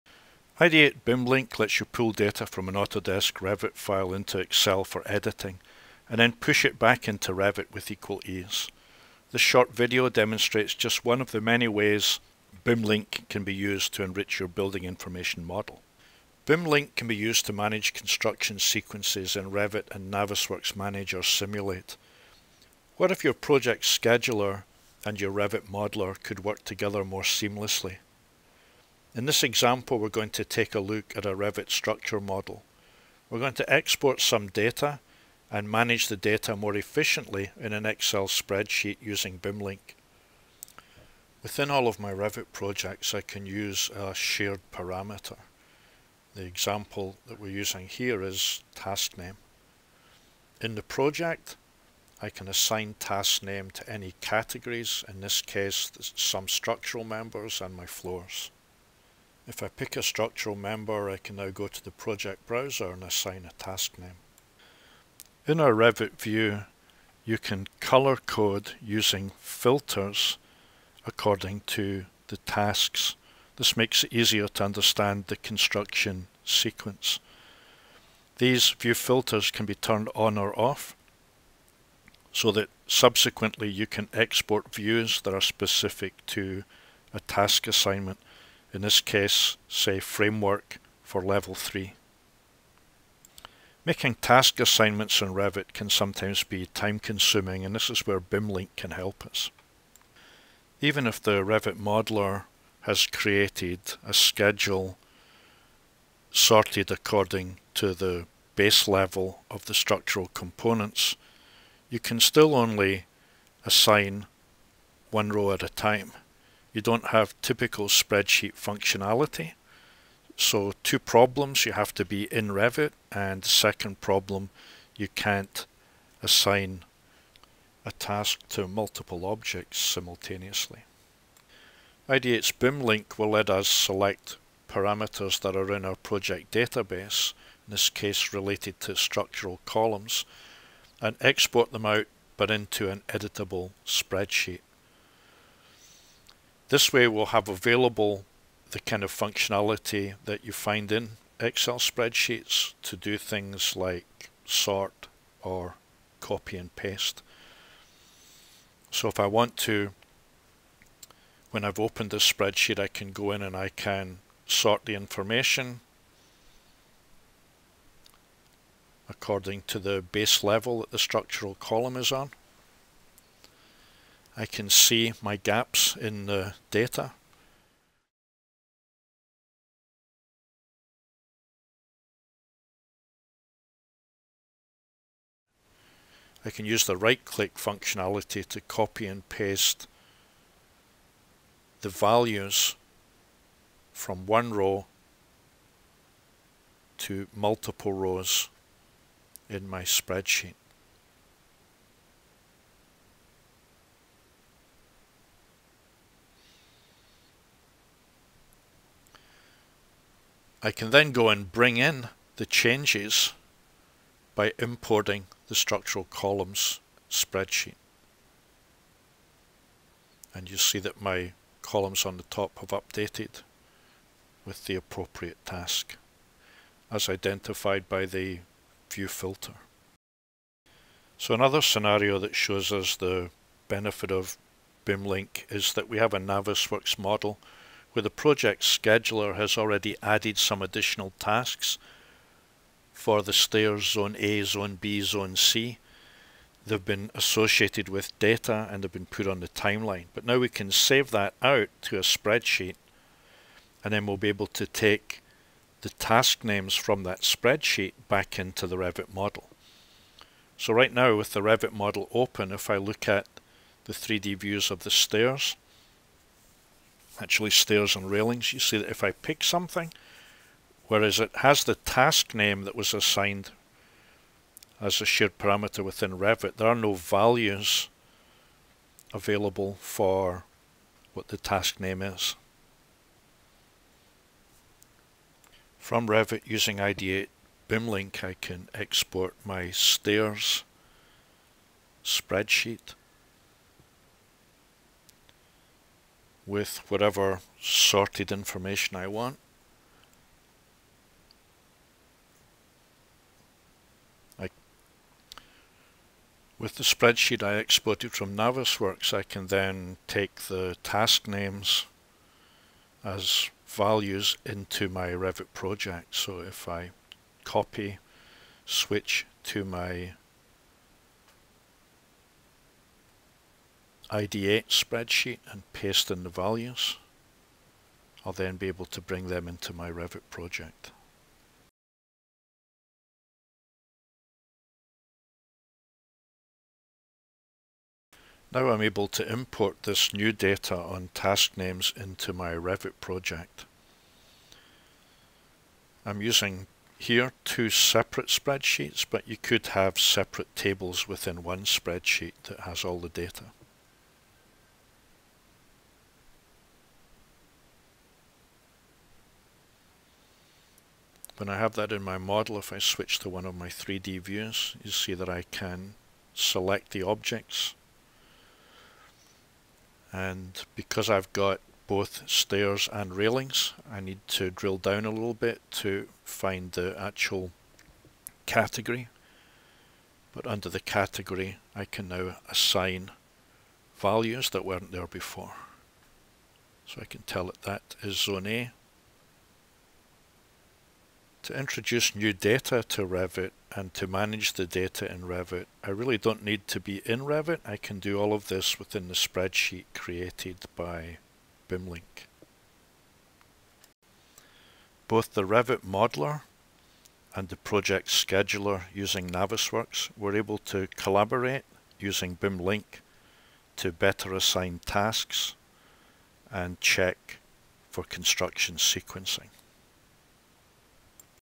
BIMLink Construction Sequencing
Use BIMLink to let you transfer data in and out of a Revit Prpject. The Revit modeler and the project scheduler can collaborate over the assignment of tasks and share information via BIMLink.